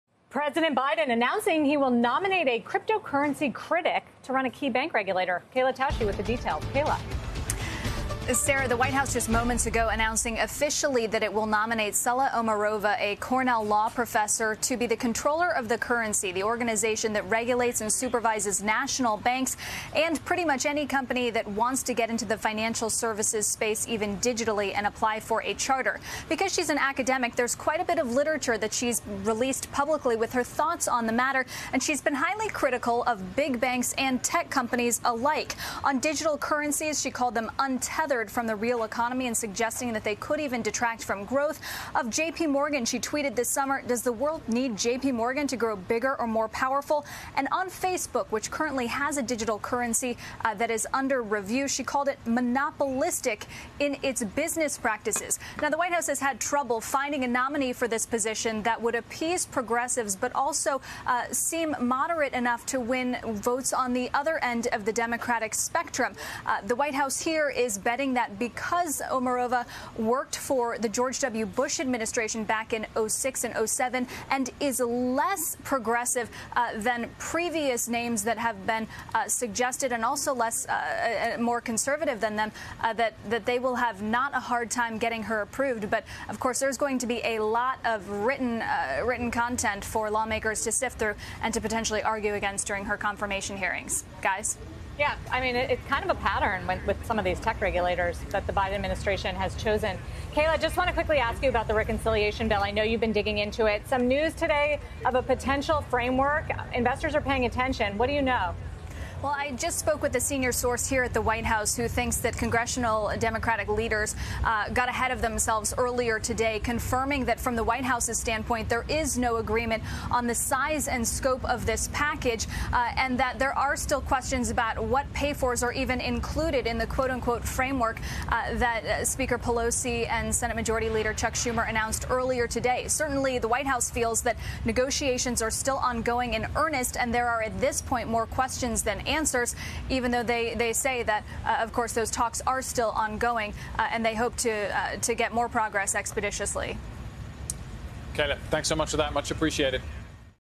Biden to nominate cryptocurrency critic to bank regulator position
Kayla Tausche joins Closing Bell with President Biden's plans to nominate Saule Omarova, a public critic of crypto, to be the Comptroller of Currency.